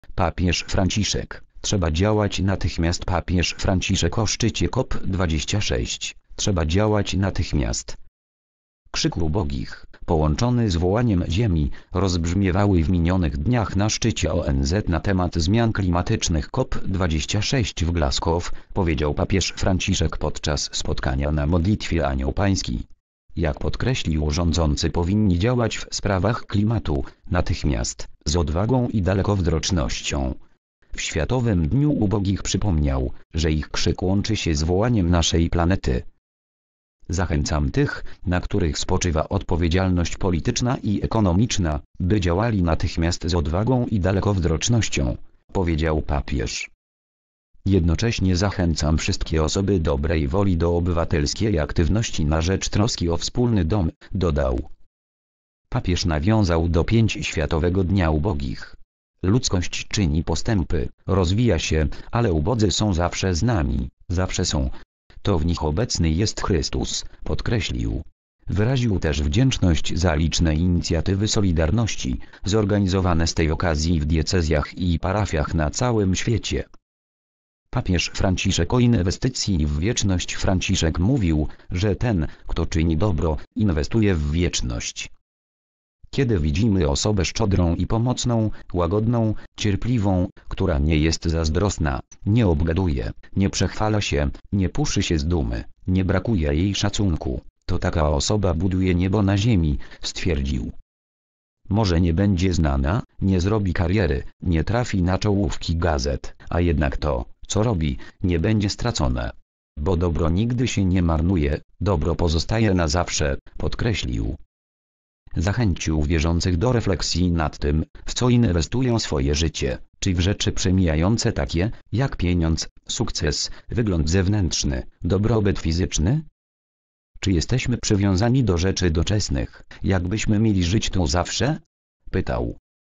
Papież Franciszek: Trzeba działać natychmiast.
Papież Franciszek o szczycie COP26: Trzeba działać natychmiast.

  (fot. PAP/EPA/Riccardo Antimiani)